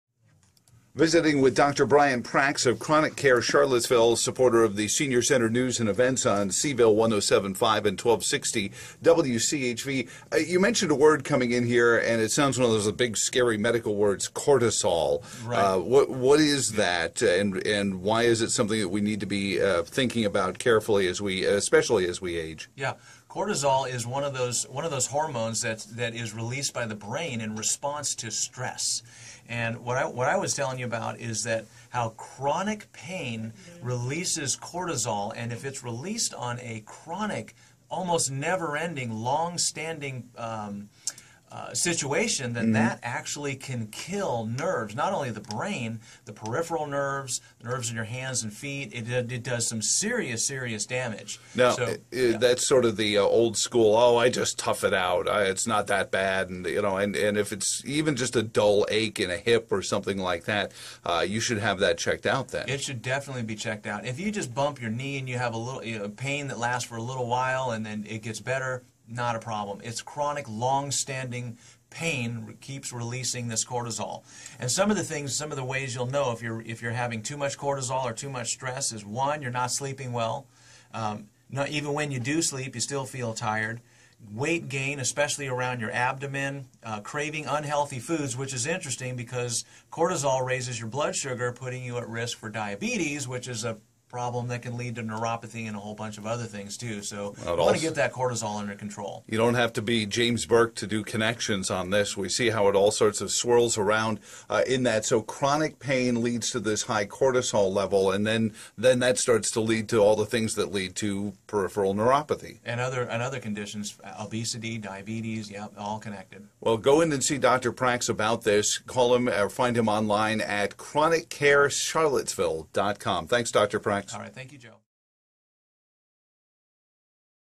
Dr. Prax's Senior Center Radio Interviews; Cortisol
Dr. Prax explains the effects of the hormone cortisol and its effects.

Are you suffering from neuropathy? Reverse your neuropathy now!